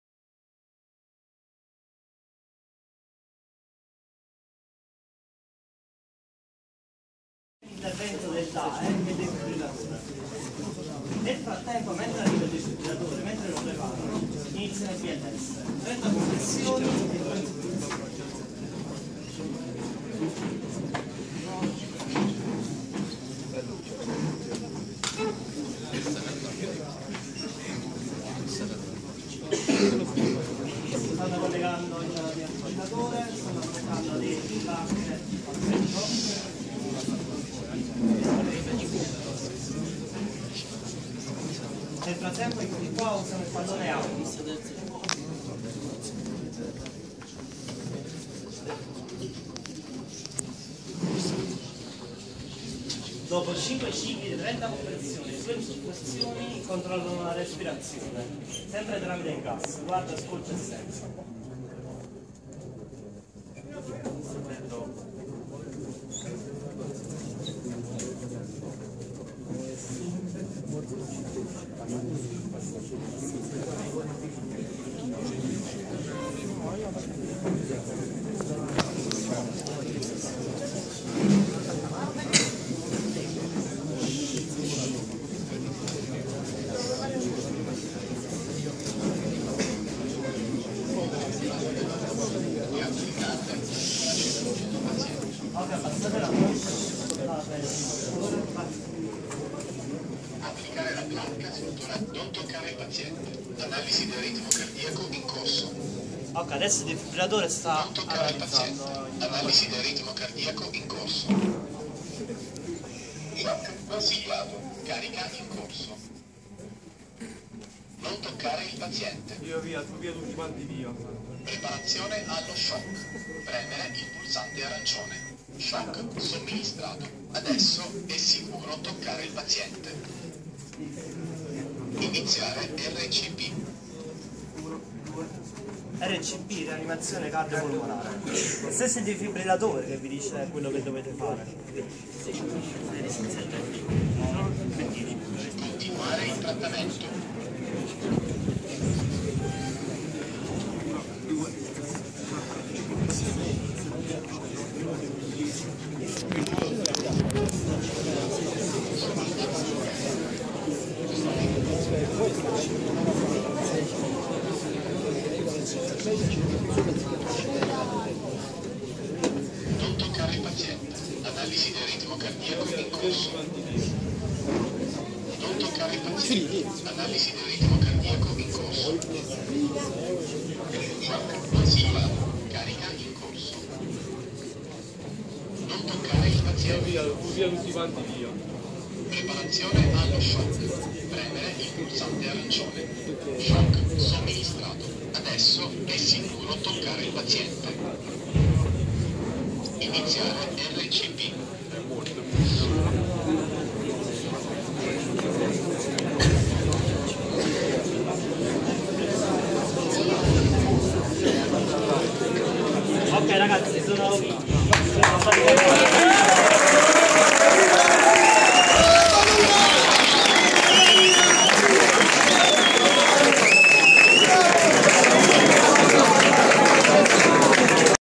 Simulazione intervento BLSD
Alla presentazione del centro di formazione BLSD dell'Auras Ischia, accreditato dalla Regione Campania, i ragazzi dell'Istituto Nautico "C Mennella", sono impegnati in una simulazione di BLS e BLSD utilizzando un defibrillatore DAE